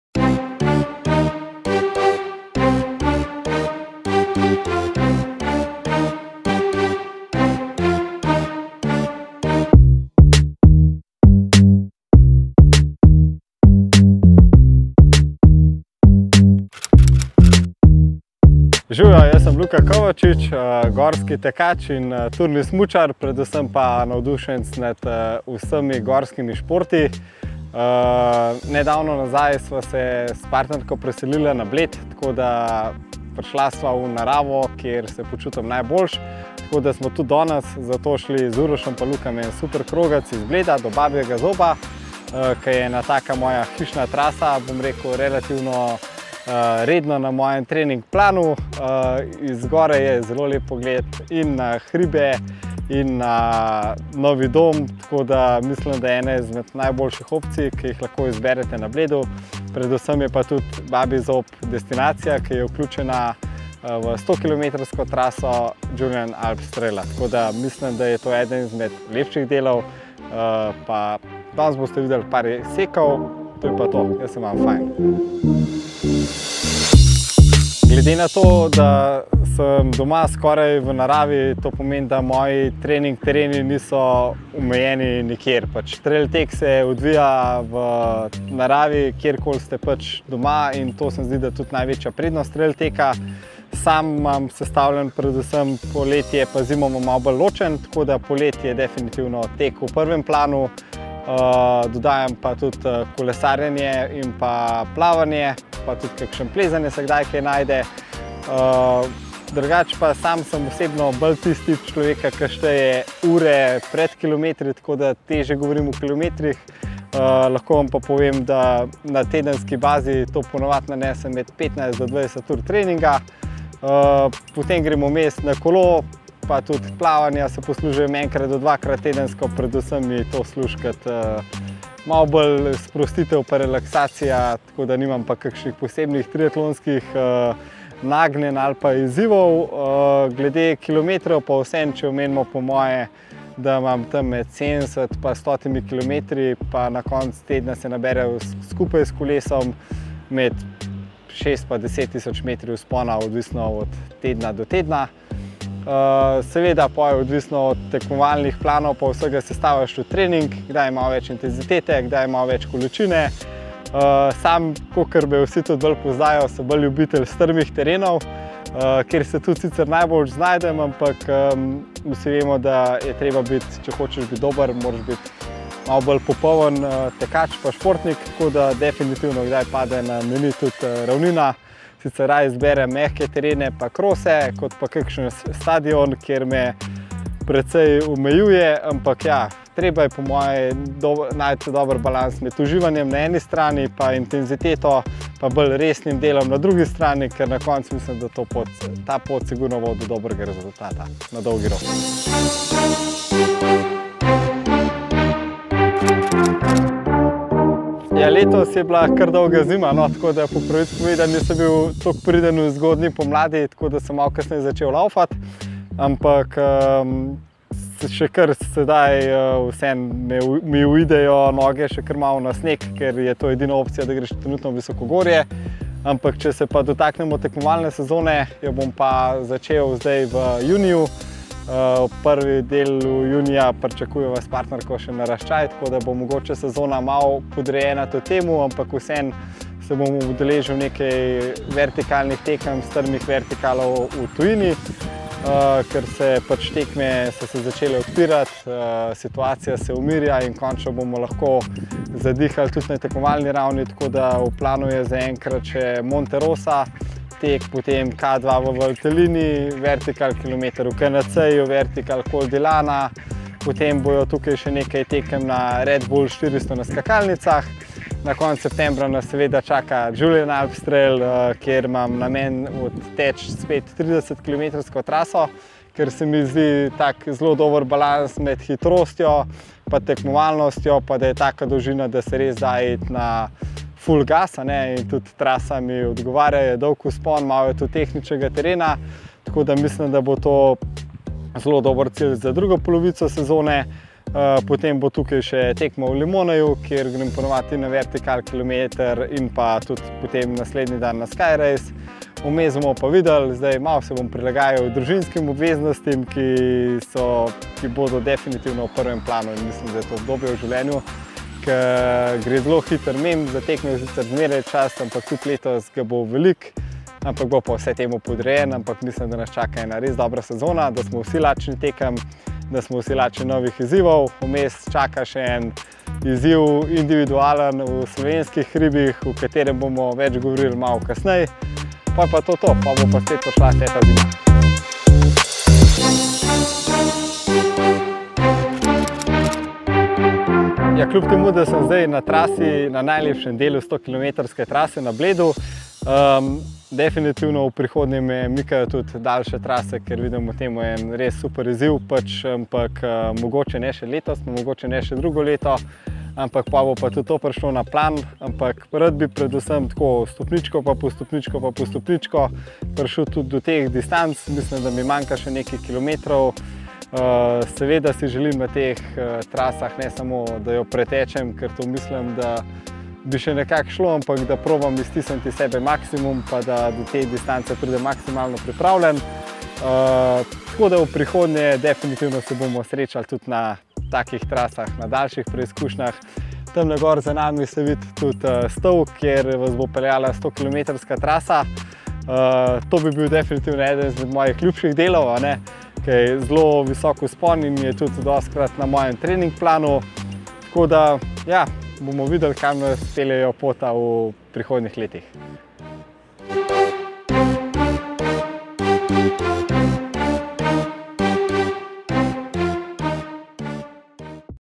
Na stezicah z LUKO KOVAČIČEM I JATrail
Na stezicah je serija pogovorov z našimi ambasadorji.

Z LUKO KOVAČIČEM smo tekli po stezicah okrog Bleda. Povzpeli smo se na Babji zob, lep razgledni hrib na robu Jelovice.Je del 100ke na Julian Alps Trail Run in Lukova redna trening trasa, zato tudi logična izbira.